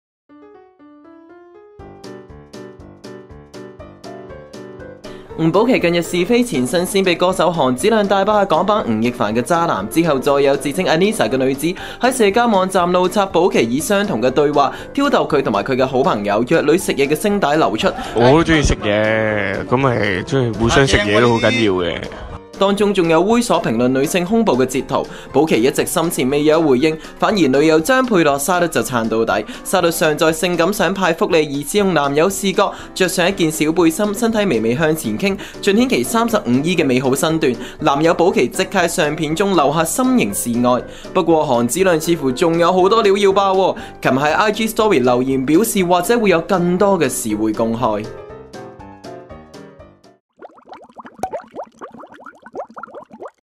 吳保錡｜女友沙律力撐 韓子亮揚言仲有嘢未爆
保錡一直深潛未有回應，反而女友張沛樂（沙律）就撐到底。